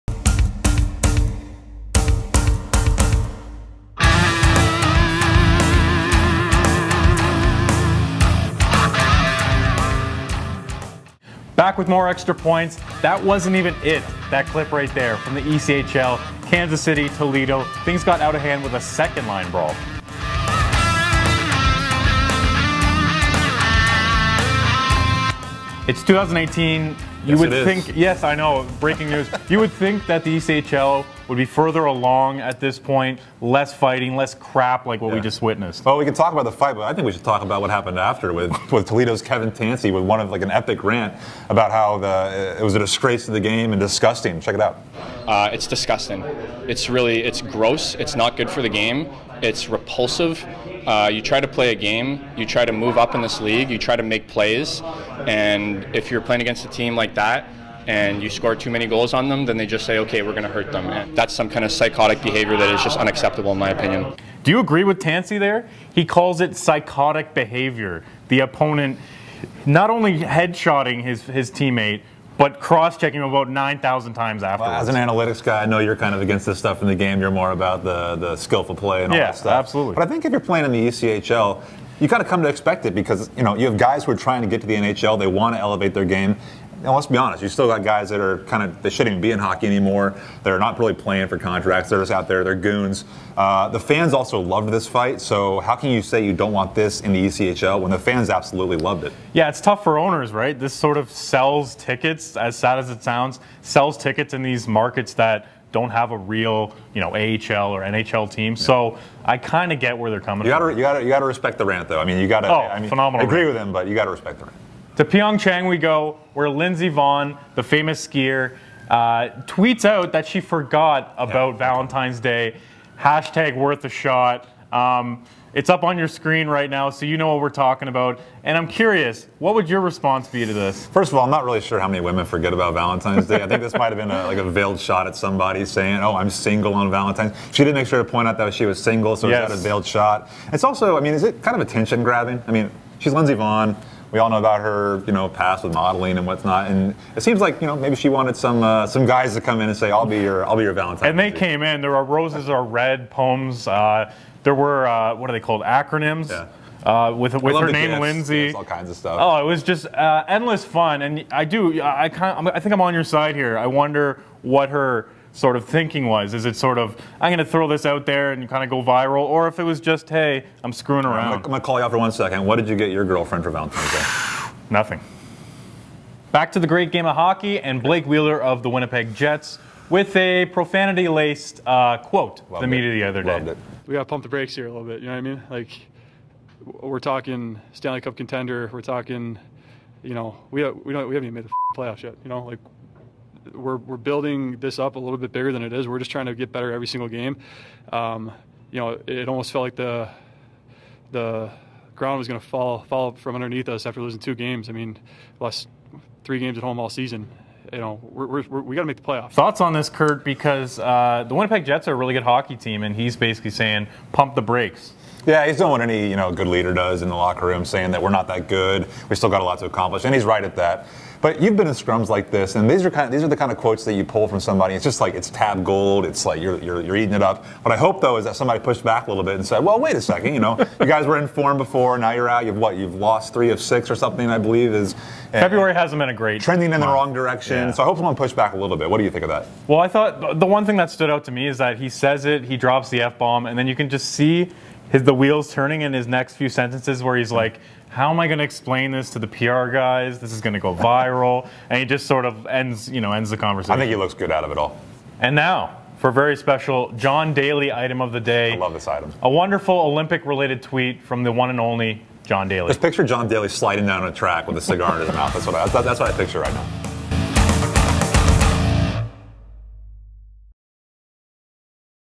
EXTRA POINTS: 'Psychotic' hockey and Vonn's lonely V-Day
Extra Points! Kurtis Larson and John Matisz break down the viral sports topics of the day.